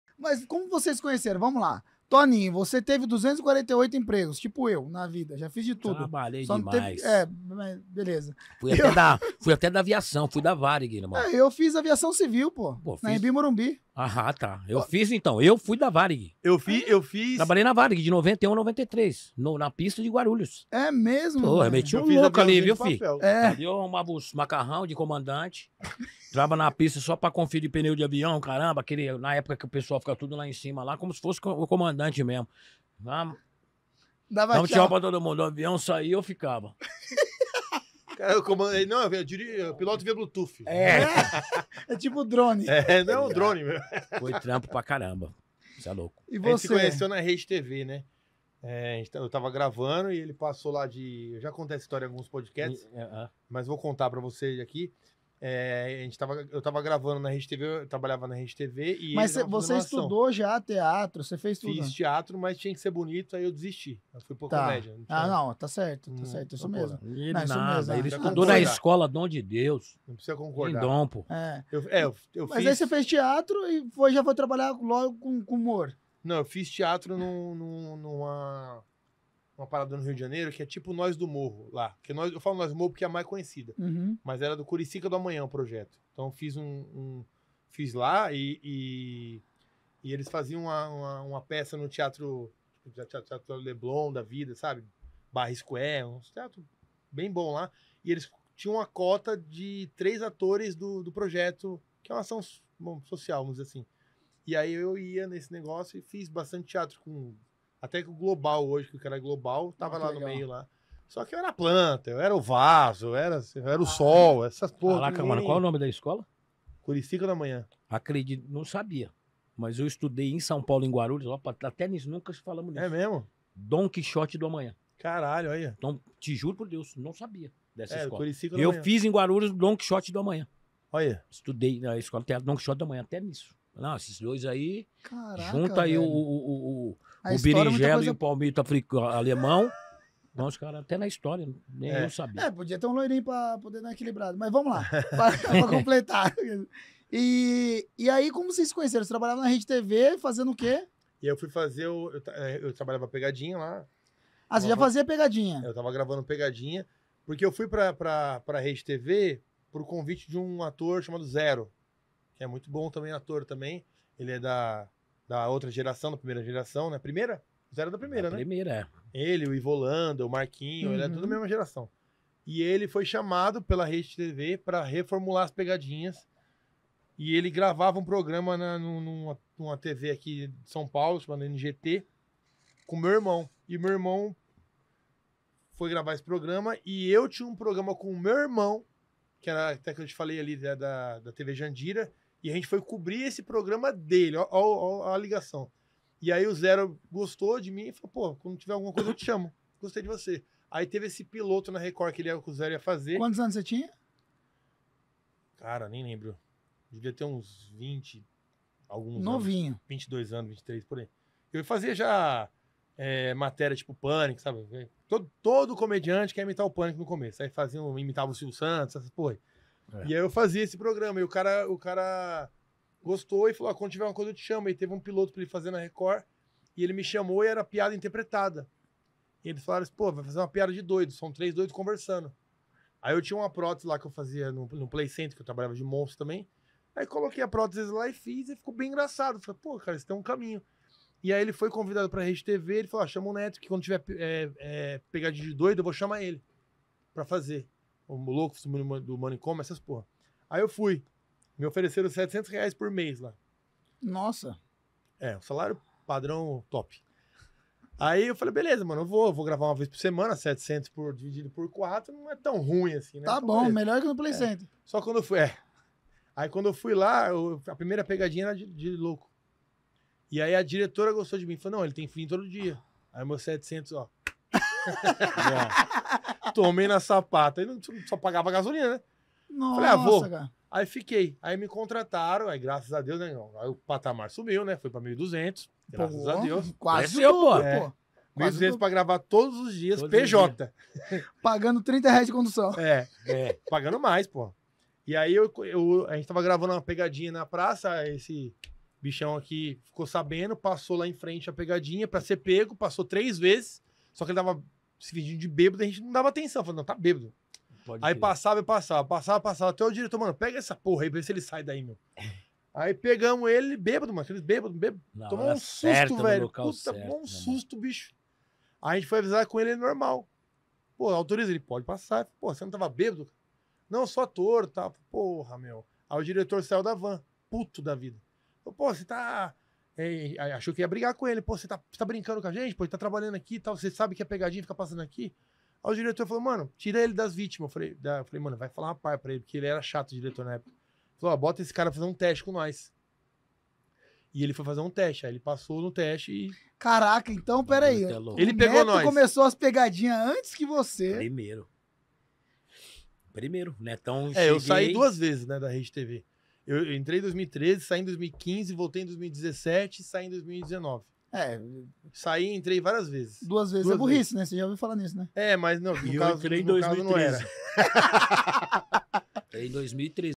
COMO NETTO TOMAZ COMEÇOU NO HUMOR (Toninho Tornado e Netto Tomaz)
Netto Tomaz conta como entrou no mundo humoristico

Léo Chile: @leochileoficial

PodVip: @podvip.podcast

Convidado:

Toninho Tornado: @toninhotornado

Netto Tomaz: @onettotomaz